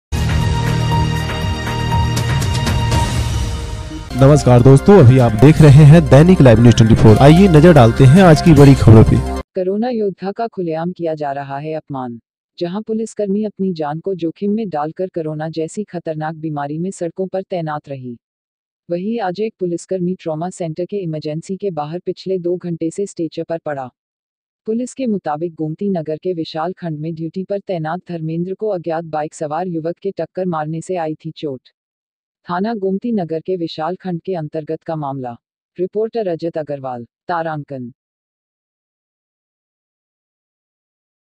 करोना योद्धा का खुलेआम किया जा रहा है अपमान।
जहां पुलिसकर्मी अपनी जान को जोखिम में डालकर करोना जैसी खतरनाक बीमारी में सड़कों पर तैनात रही

*वही आज एक पुलिसकर्मी ट्रामा सेंटर के इमरजेंसी के बाहर पिछले 2 घंटे से स्टेचर पर पड़ा*

पुलिस के मुताबिक गोमतीं नगर के विशाल खंड में ड्यूटी पर तैनात धर्मेन्द्र को अज्ञात बाइक सवार युवक के टक्कर मारने से आई थी चोट

*थाना गोमती नगर के विशाल खंड के अंतर्गत का मामला*

 *रिपोर्टर रजत अग्रवाल।।*